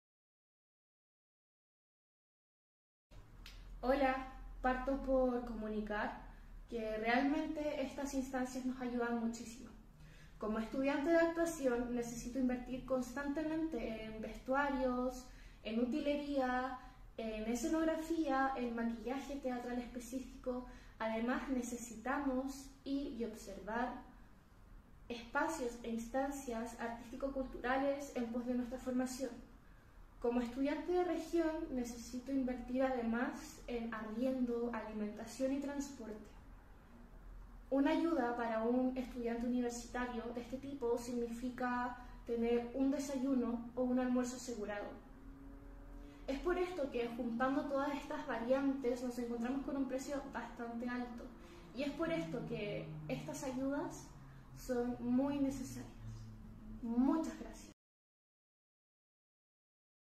Ayelén Soto - Giving Day 2023
Como Facultad de Artes UC, tenemos el desafío de aumentar el apoyo de becas de manutención estudiantil para continuar forjando futuros creadores ¡Te invitamos a que nos ayudes a ayudar!